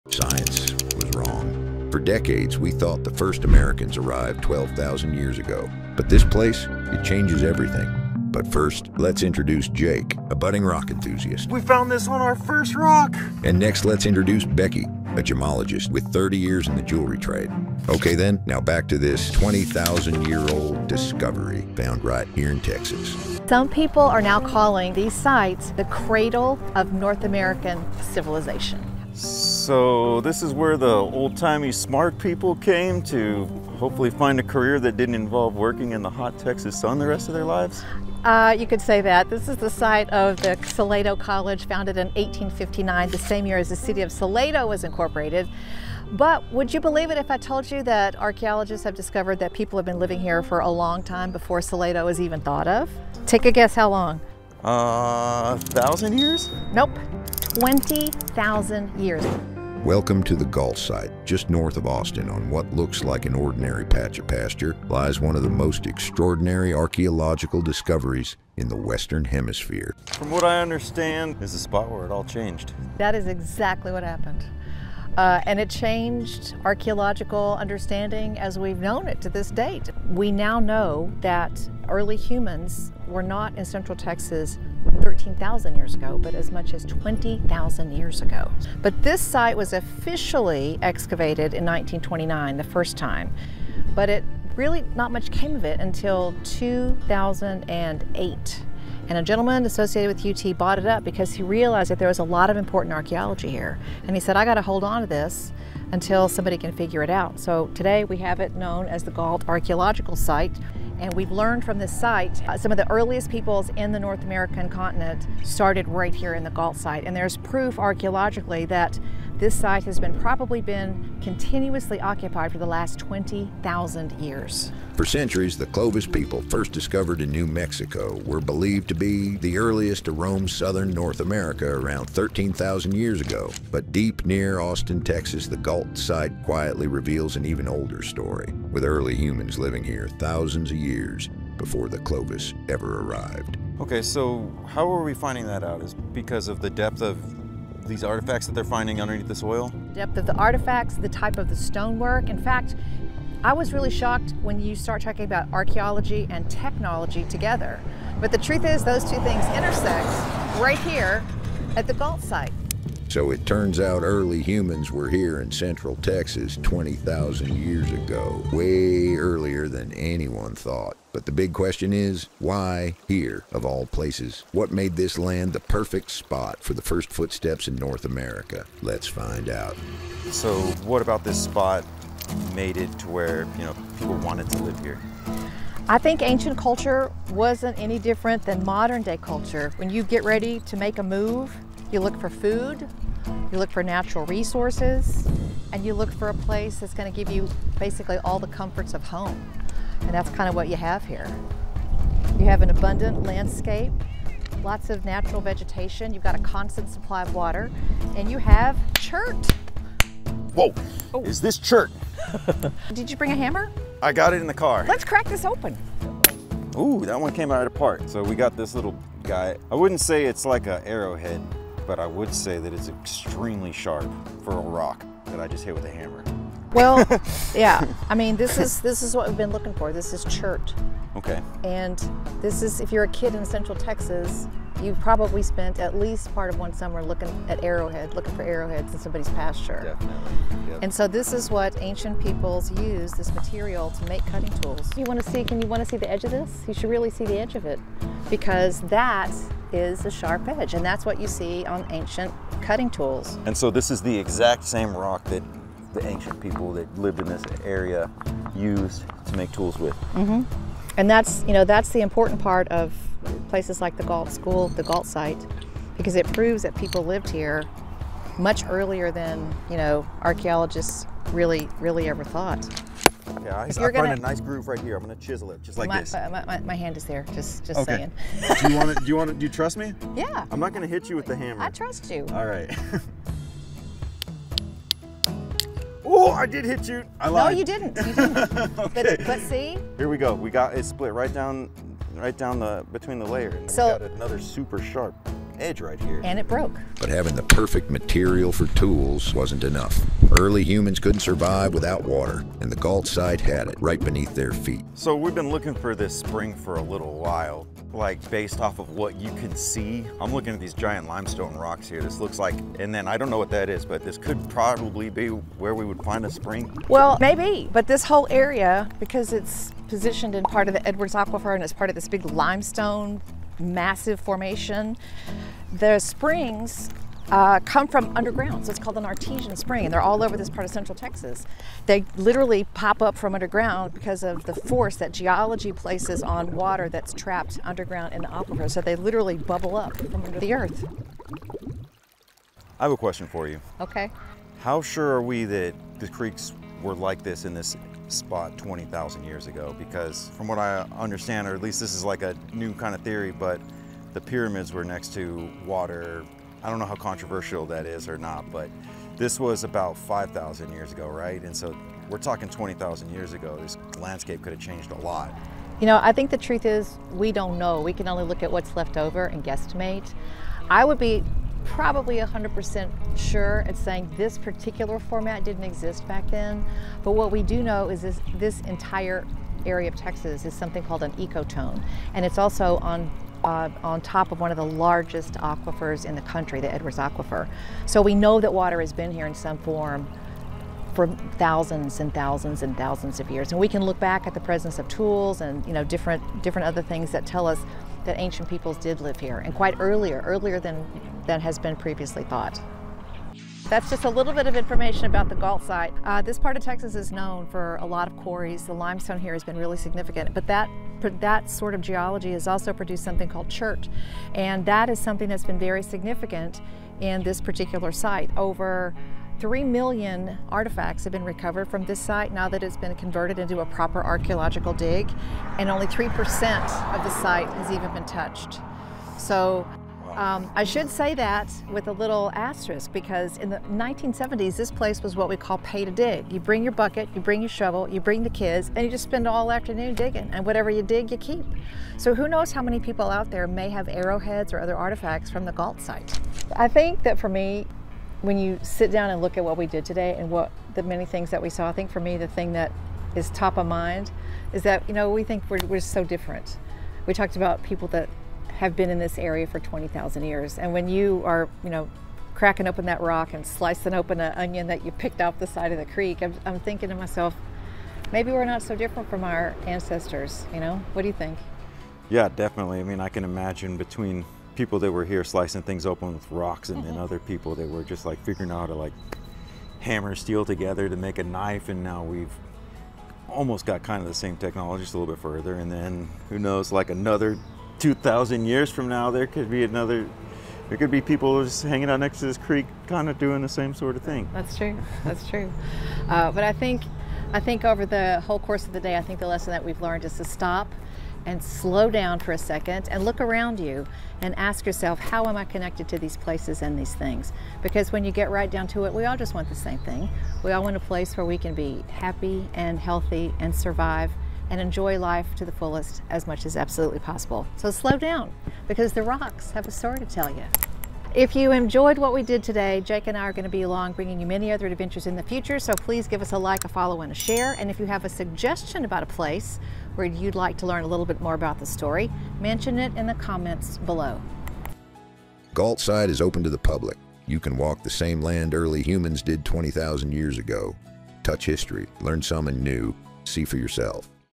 New Evidence of First Americans: Pre-Clovis People in Texas (2025) | Gault Site
🔍 Science Was Wrong

For decades, archaeologists believed the first Americans arrived around 12,000 years ago. But deep beneath the Texas soil lies a discovery that changes everything: the Gault Site, a place now called the cradle of North American civilization. In this video, join Jake and Becky—a rockhound and seasoned gemologist—as they unearth evidence that people lived in central Texas 20,000 years ago, rewriting both American history and Native American history.

• Why the Gault archaeological site challenges the old Clovis-first model
• How archaeologists dated stone tools and artifacts buried 20,000 years deep
• The significance of chert stone tools and their evolution over 7,000 years
• What the Edwards Aquifer and Salado Creek reveal about ancient lifeways
• Insights into how early humans in North America survived, hunted, and thrived

🏺 Why Gault Matters

Before Salado College or even the incorporation of Salado in 1859, people gathered here—drawn by springs bubbling from the Edwards Aquifer, abundant chert for toolmaking, and plentiful game. The depth and complexity of artifacts at the Gault Site prove continuous occupation for millennia, altering our understanding of Native American migration and innovation.

🌱 From Rock to Story

Watch as Becky cracks open ancient chert, revealing the razor-sharp edge early humans used to hunt and process food. Stand on Salado Creek’s banks and feel connected to ancestors who made tools with their own hands—20,000 years before scientists thought anyone lived here.

0:00 Meet Becky and Jake
1:00 History of the Gault Site
2:05 Clovis and Pre-Clovis People
2:55 Why people came here
3:35 Chert Tool Making
5:05 The Edwards Aquifer
6:49 Quarries next to the Gault Site
8:00 What we learned